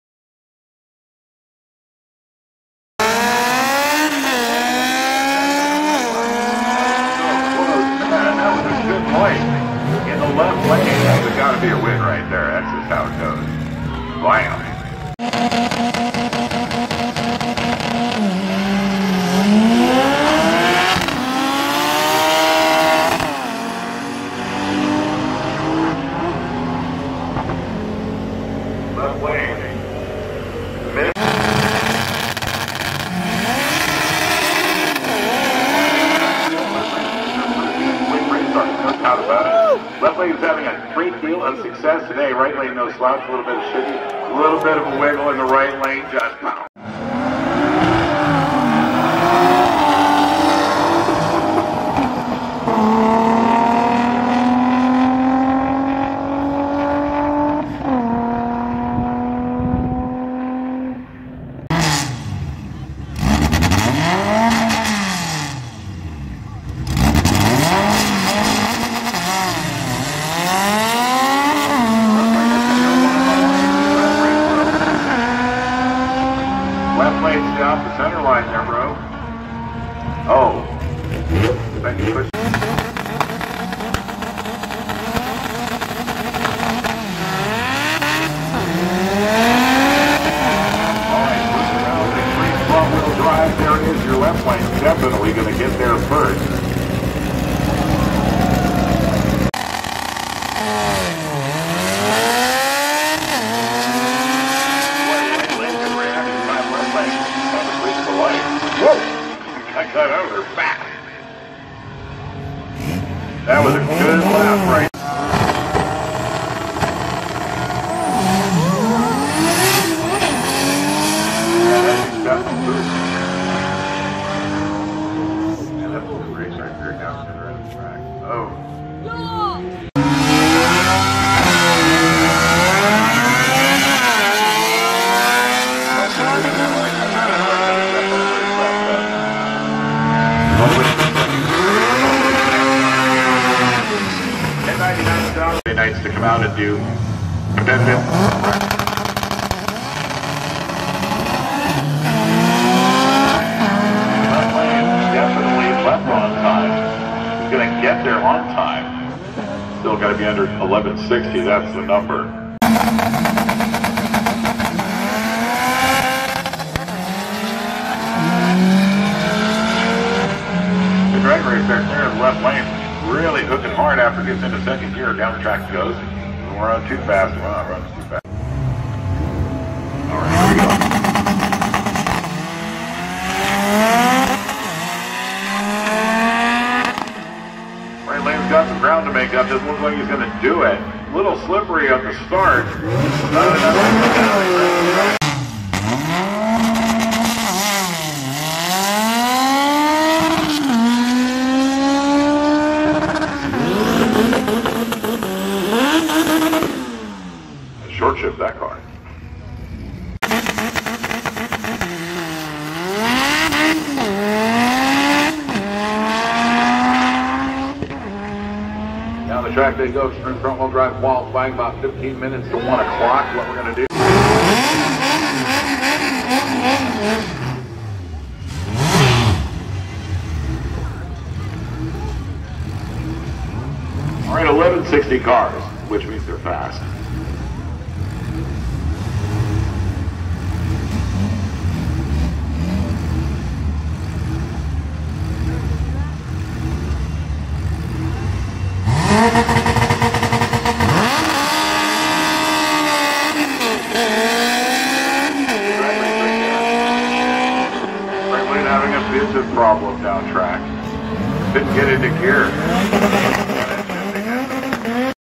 Sacramento Raceway CMI Event Fall Fling Finals September 23,2020
Small clips racing event @ Sacramento raceway. Cmi drags fall fling finals